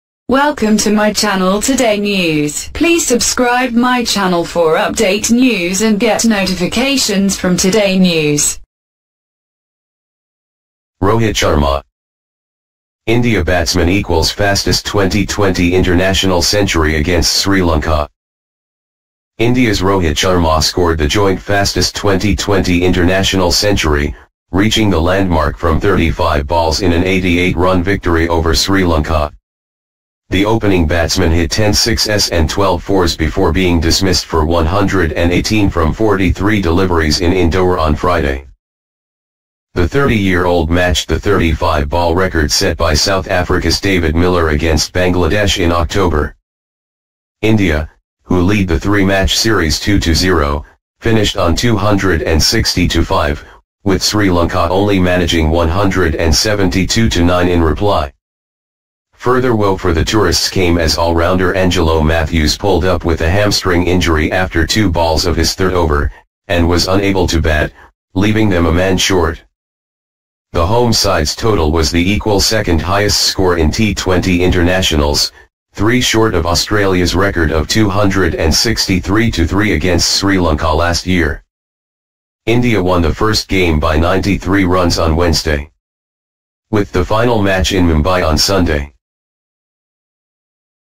Rohit Sharma: India batsman equals fastest Twenty20 international century against Sri Lanka
Rohit Sharma: India batsman equals fastest Twenty20 international century against Sri Lanka 2017